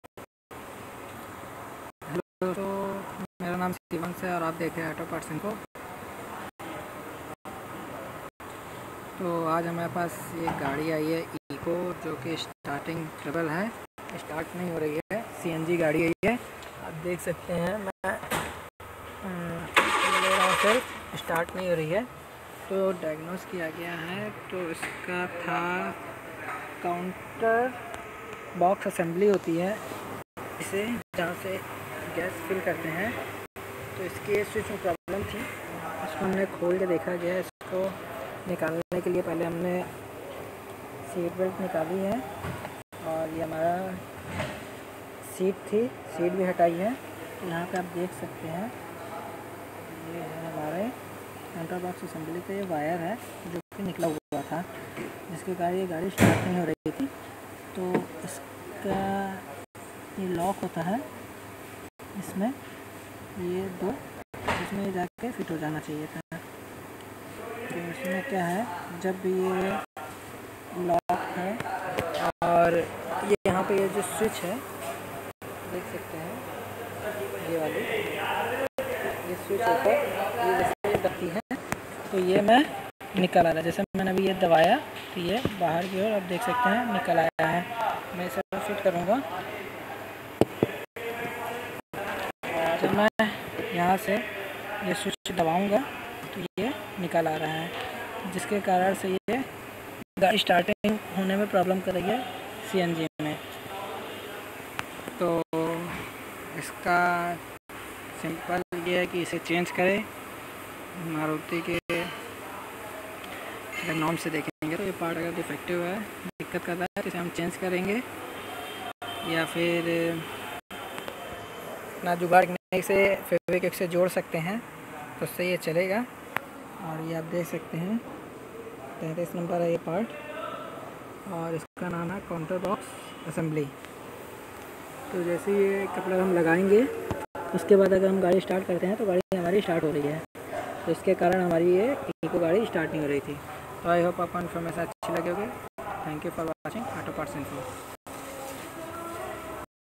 CNG Vehicle not starting problem trouble shooting in MARUTI SUZUKI ECCO
Defective counter box assembly